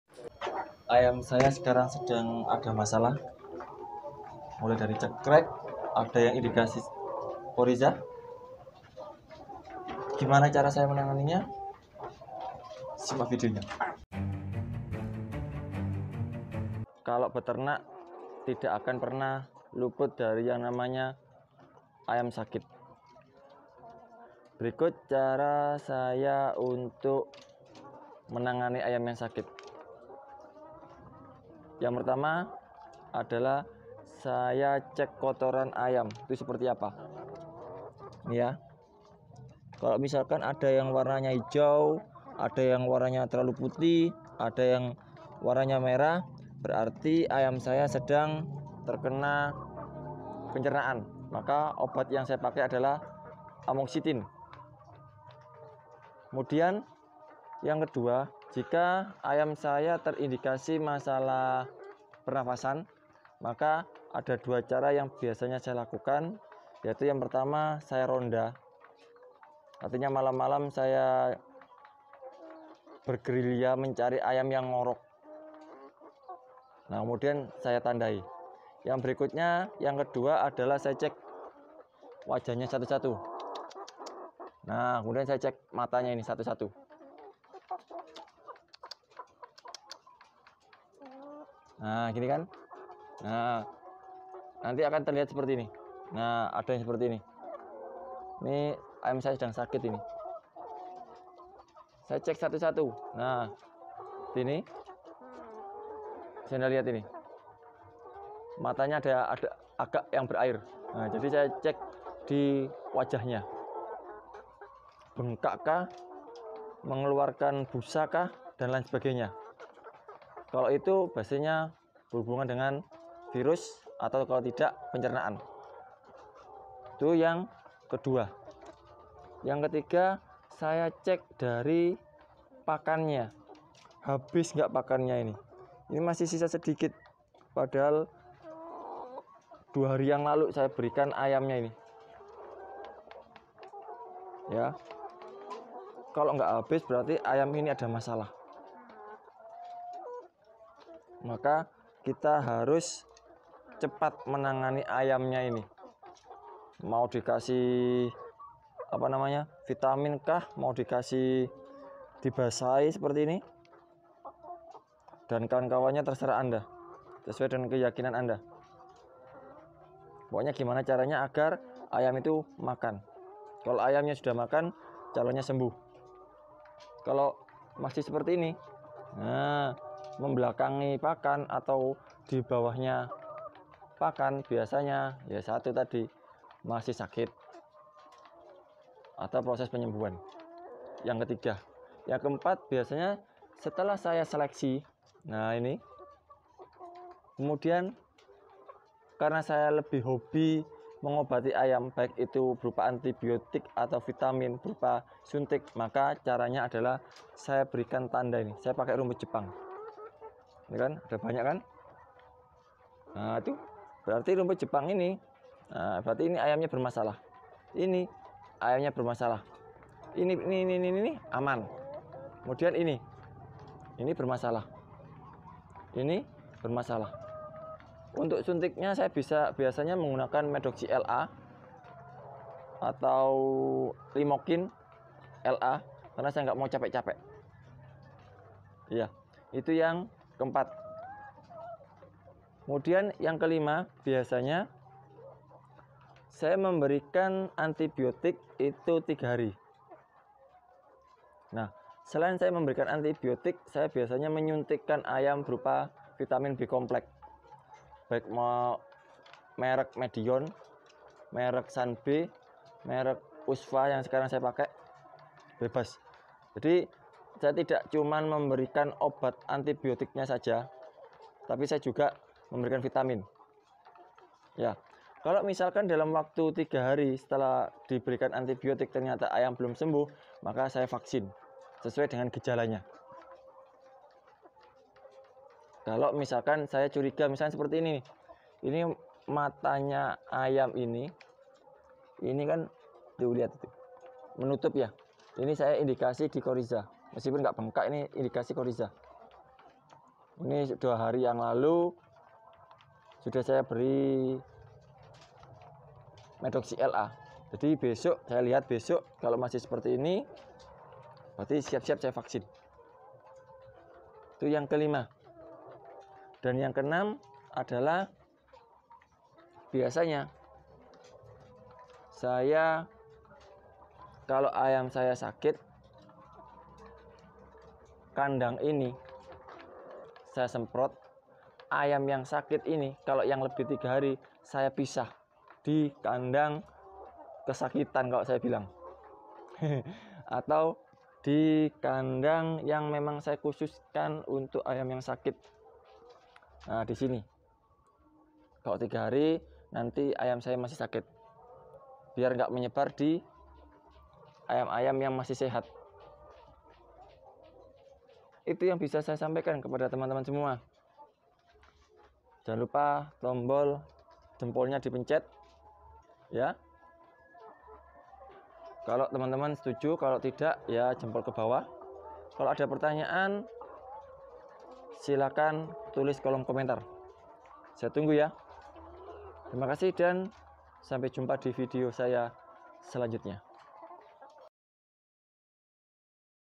cara menangani ayam sakit - simpel dan mudah
cara menangani ayam sakit yang simpel dan mudah diterapkan oleh semua peternak ayam dalam hal menangani ayam sakit di kandang yaitu kita harus tahu caranya